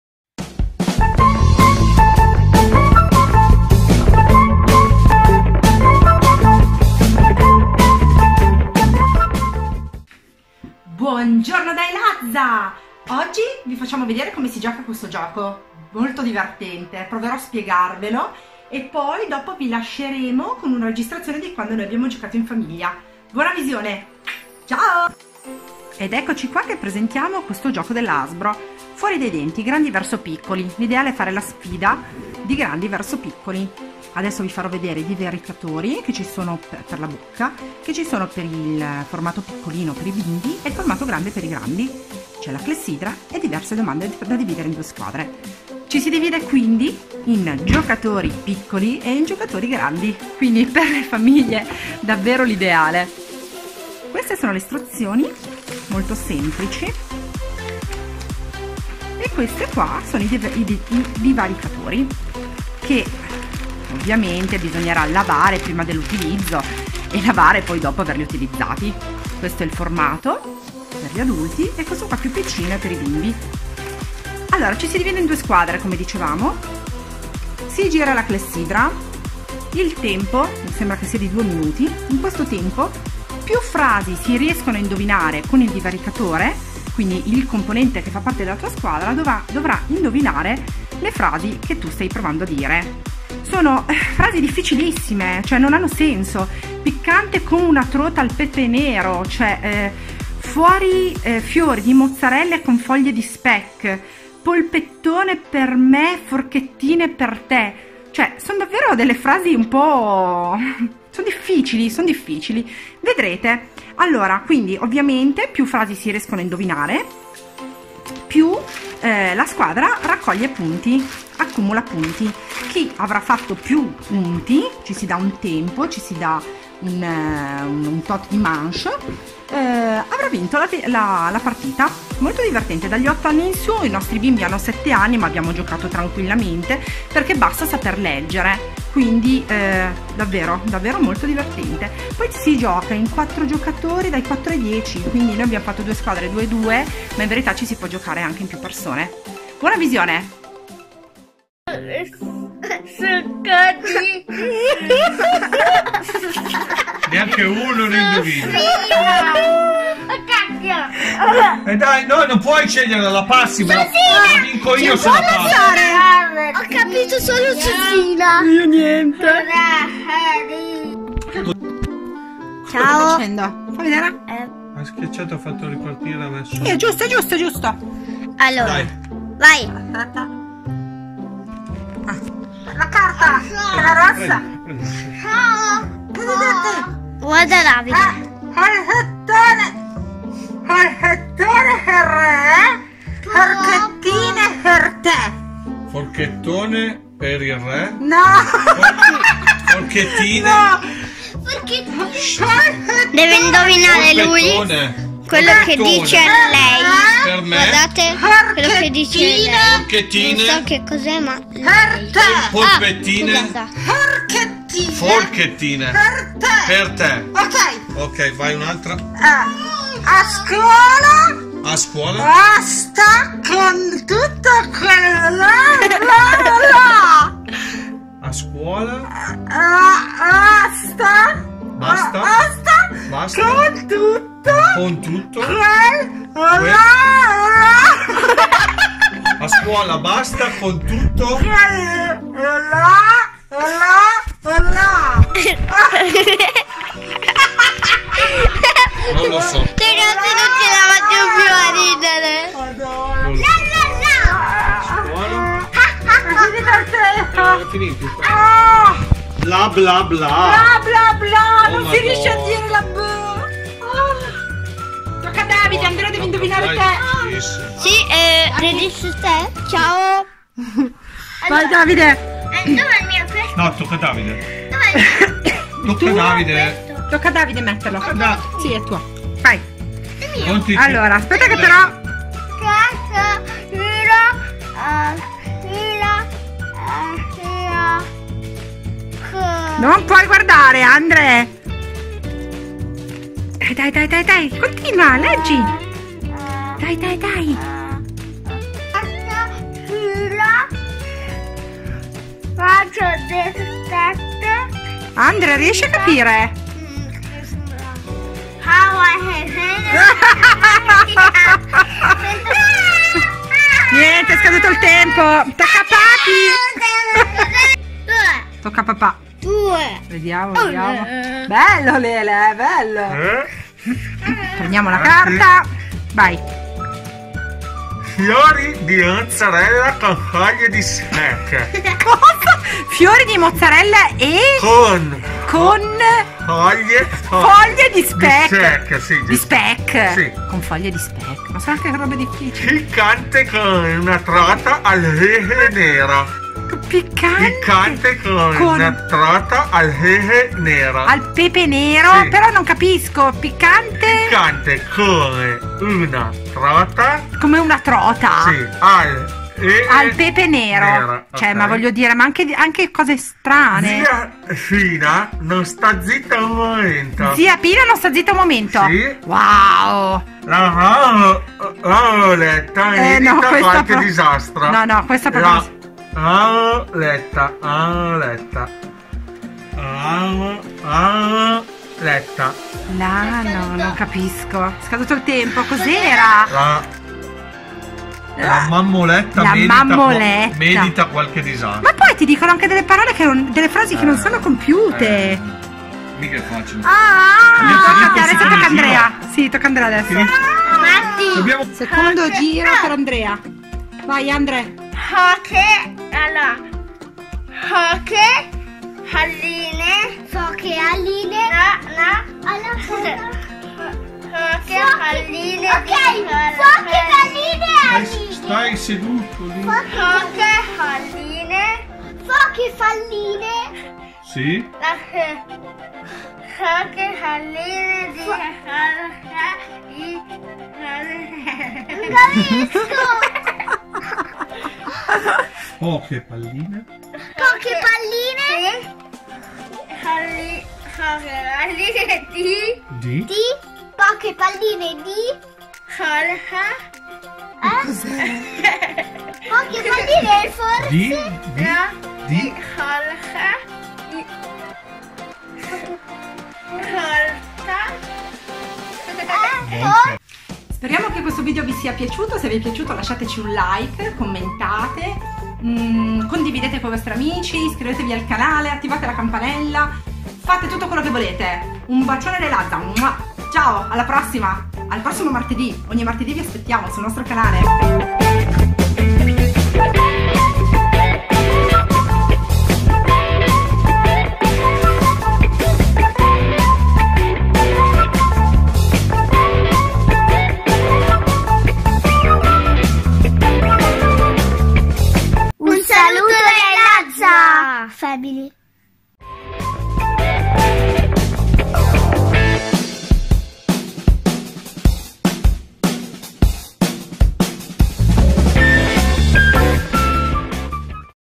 FUORI DA DENTI HASBRO
Benvenuti a casa dei lazza family
Oggi vi presentiamo un gioco davvero super divertente
Si chiama Fuori dai denti della Hasbro
buona visione
Lazza family